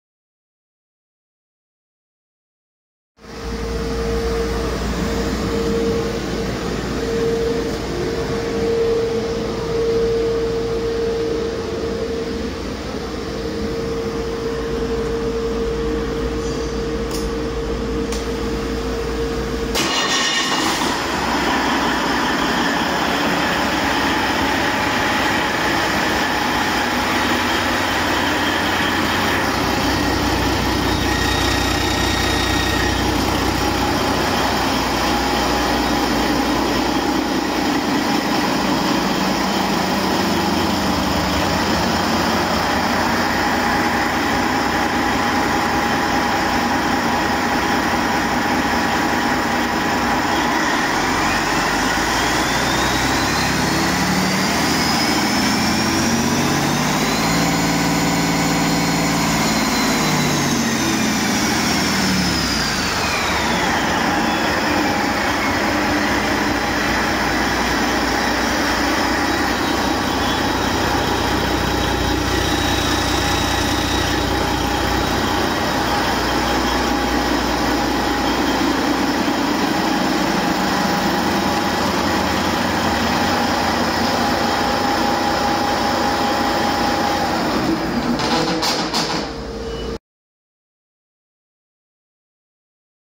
Used 2000 Detroit Diesel Series 60 DDEC IV 12.7L Engine Test Run 500-17720
Good 2000 Detroit Diesel Series 60 DDEC IV 12.7L being test run. Engine Serial Number: 06RE115863 with 430 hp. Our Stock Number is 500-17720.

Frontier Truck Parts has one of the largest inventories of Engines & Engine Parts in the Midwest.